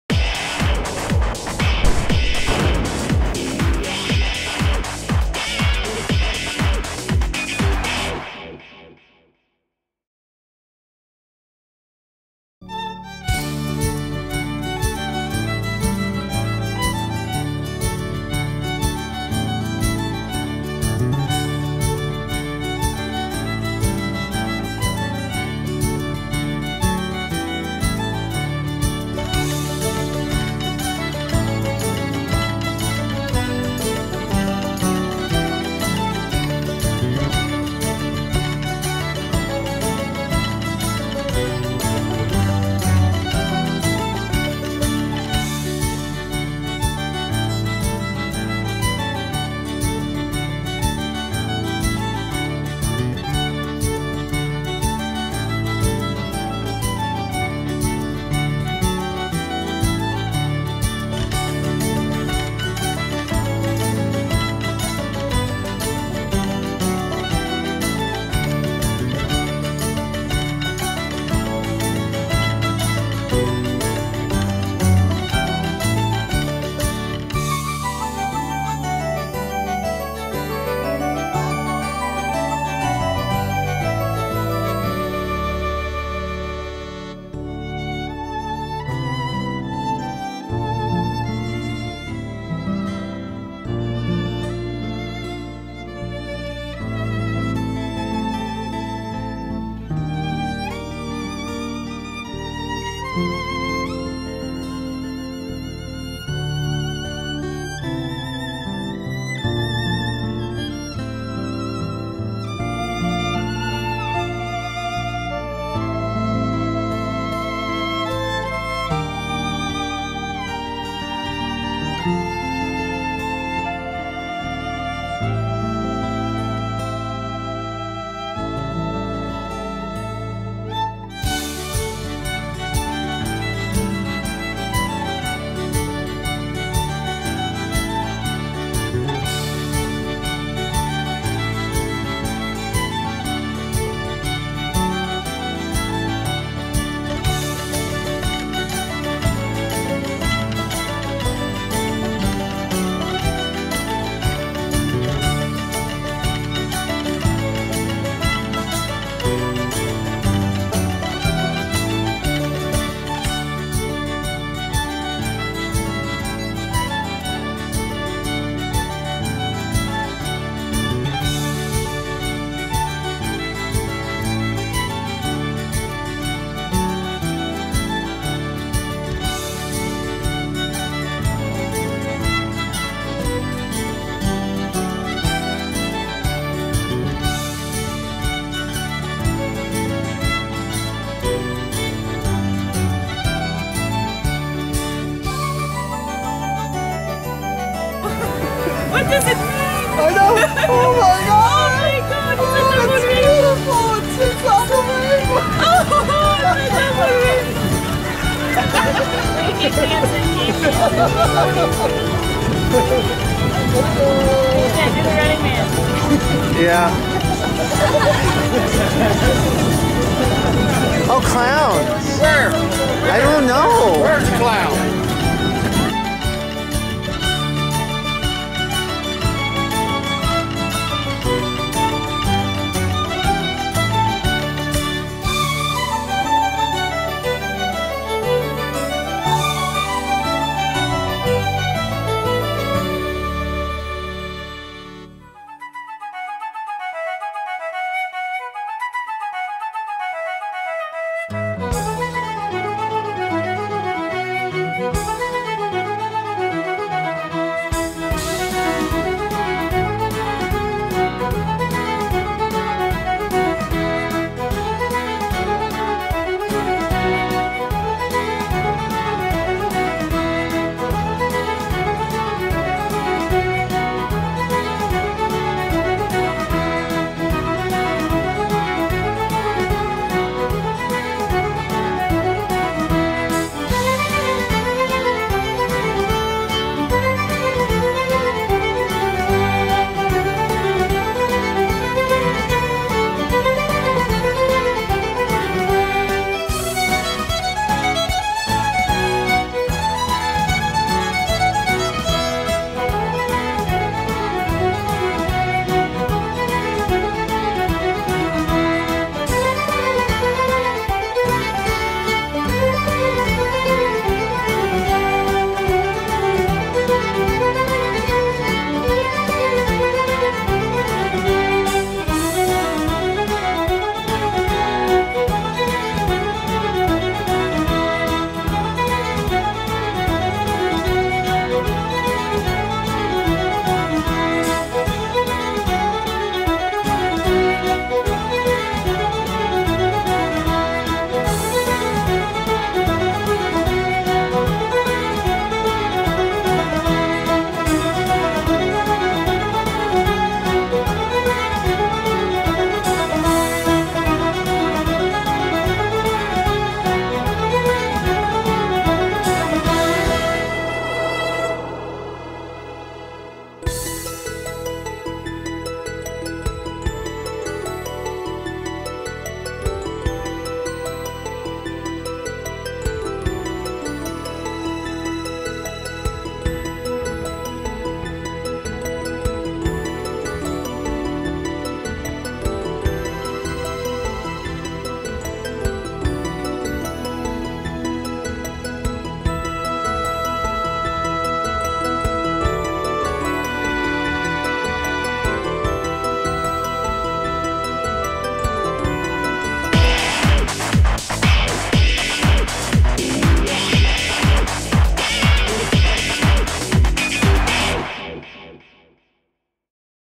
The Costume Vault In The Dublin St. Patrick's Day Parade
This year we entered a float into Dublin's annual St. Patrick's Day parade.  Here's our appearance along with a behind the scenes look at the making of our float.  All of the looks we are wearing and more are available at our store.